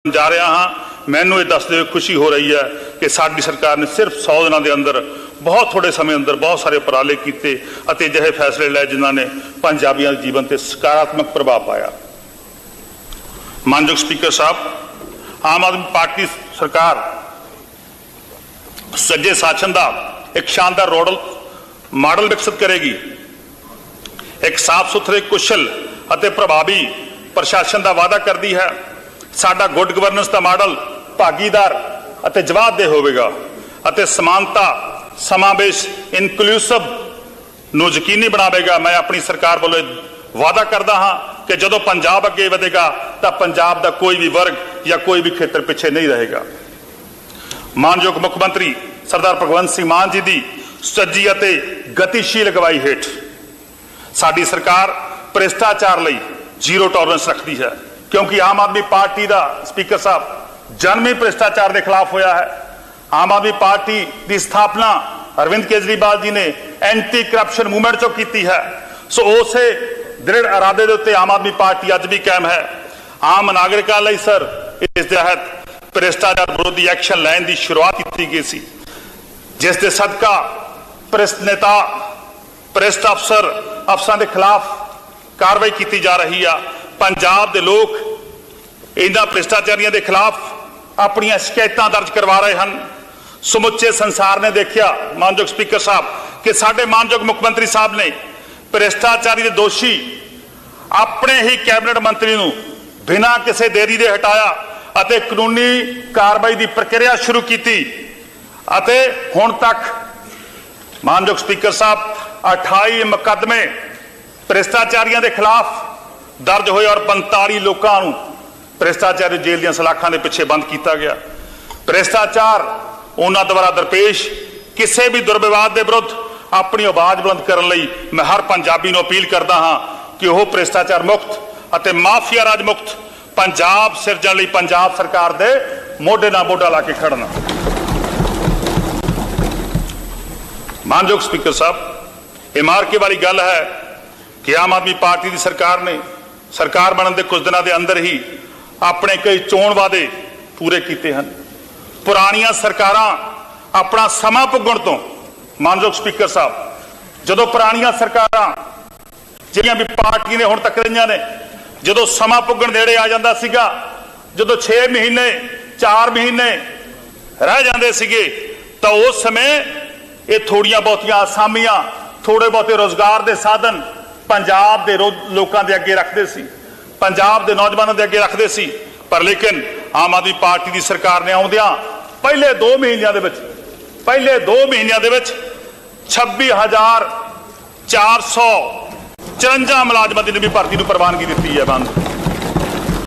Punjab Budget 2022 : 100 ਦਿਨਾਂ ਅੰਦਰ ਸਰਕਾਰ ਨੇ ਕਈ ਅਹਿਮ ਫ਼ੈਸਲੇ ਲਏ- Harpal Cheema | News18 Punjab
Finance Minister Harpal Cheema ਨੇ ਪੰਜਾਬ ਦਾ ਬਜਟ ਪੇਸ਼ ਕਰ ਰਹੇ। ਉਨ੍ਹਾਂ ਕਿਹਾ ਕਿ ਪੰਜਾਬ ਨੇ ਲੋਕਾਂ ਨੇ ਬਦਲਾਅ ਲਈ ਵੋਟਾਂ ਪਾਈਆਂ।  ਉਨ੍ਹਾਂ ਕਿਹਾ ਕਿ ਪੰਜਾਬ ਦਾ ਬਜਟ ਸ਼ਹੀਦਾਂ ਦੇ ਸੁਫ਼ਨਿਆਂ ਨੂੰ ਪੂਰਾ ਕਰਨ ਵਾਲਾ ਬਜਟ ਹੋਵੇਗਾ। ਉਨ੍ਹਾਂ ਇਹ ਵੀ ਕਿਹਾ ਕਿ ਭ੍ਰਿਸ਼ਟਾਚਾਰ ਤੇ ਮਾਫ਼ੀਆ ਰਾਜ ਨੂੰ ਖਤਮ ਕਰਾਂਗੇ। ਉਨ੍ਹਾਂ ਇਹ ਵੀ ਕਿਹਾ ਕਿ ਆਮ ਆਦਮੀ ਪਾਰਟੀ ਨੇ100 ਦਿਨਾਂ ਅੰਦਰ ਸਰਕਾਰ ਨੇ ਕਈ ਅਹਿਮ ਫੈਸਲੇ ਲਏ ਤੇ ਪੰਜਾਬ ਨੂੰ ਖੁਸ਼ਹਾਲ ਸੂਬਾ ਬਣਾਵਾਂਗੇ। ਉਨ੍ਹਾਂ ਕਿਹਾ ਕਿ ਪੰਜਾਬ ਅੱਗੇ ਵਧੇਗਾ ਤੇ ਕੋਈ ਵੀ ਵਰਗ ਪਿੱਛੇ ਨਹੀਂ ਰਹੇਗਾ।